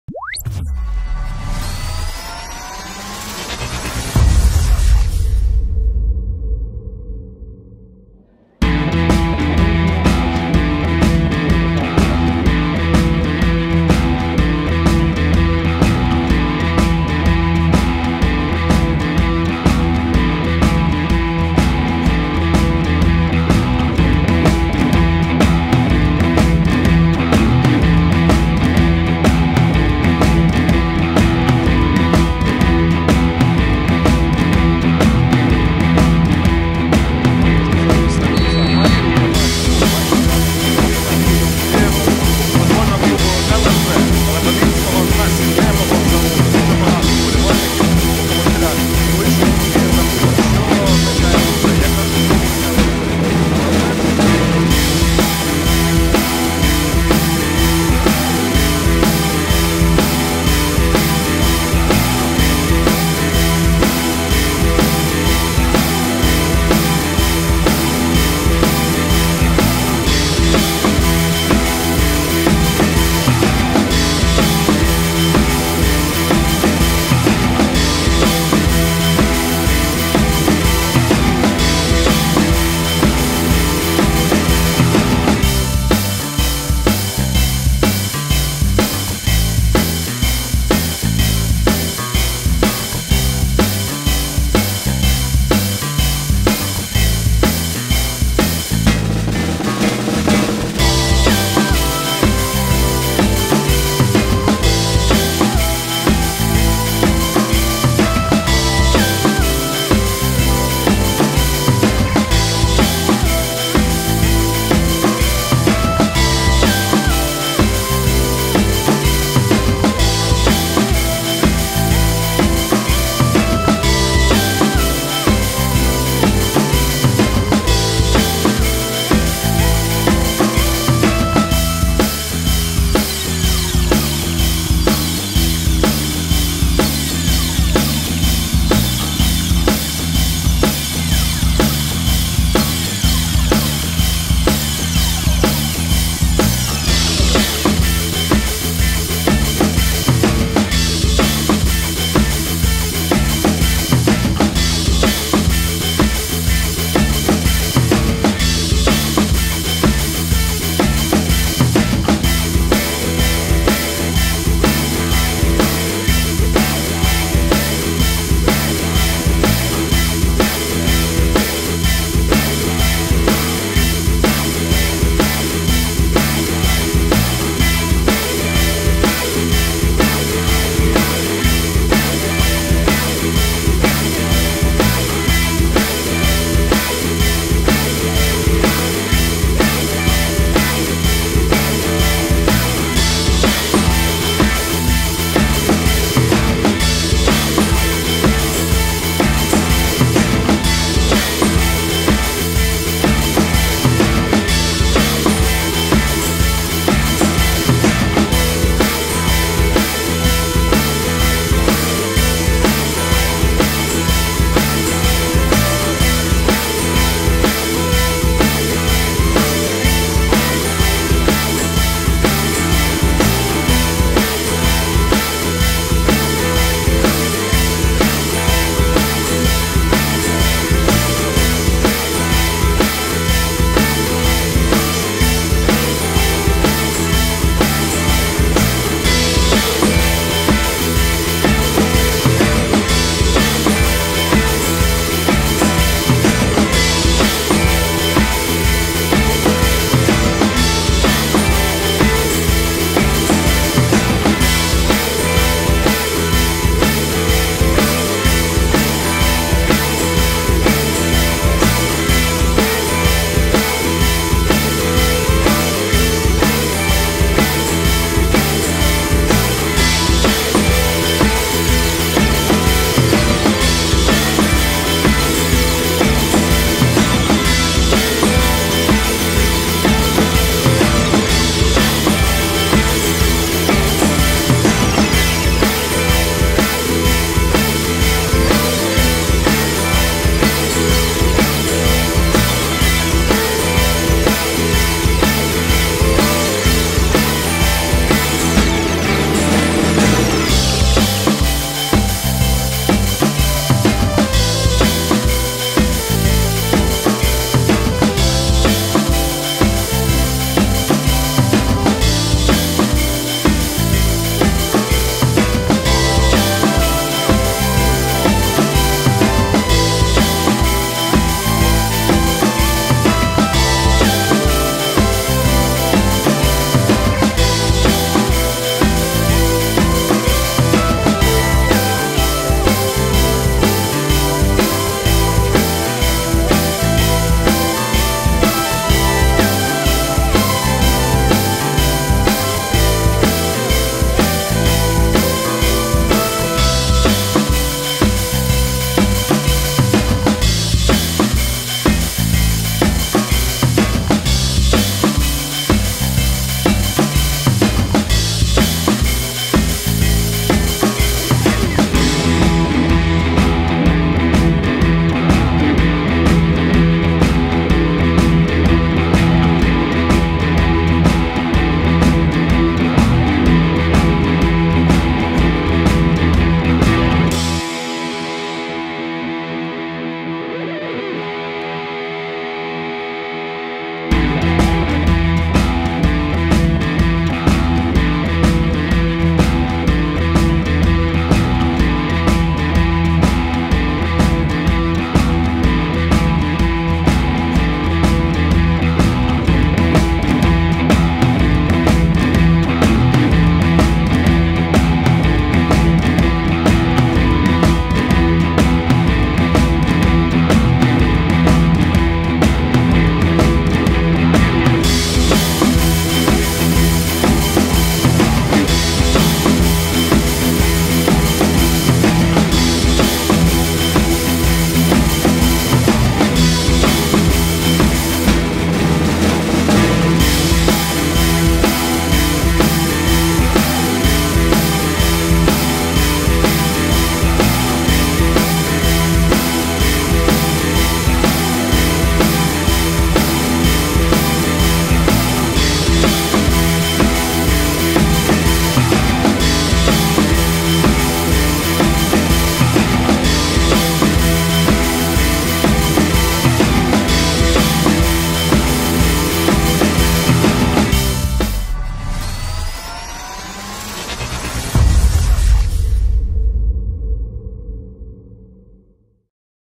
Л.Н.З. 2:1 Олимпик (31.08.2021) Обзор матча 3 предварительного этапа Кубка Украины 2021/2022
Л.Н.З. 2:1 Олимпик. Смотрите обзор матча Кубка Украины 2021/2022 от OlympicTV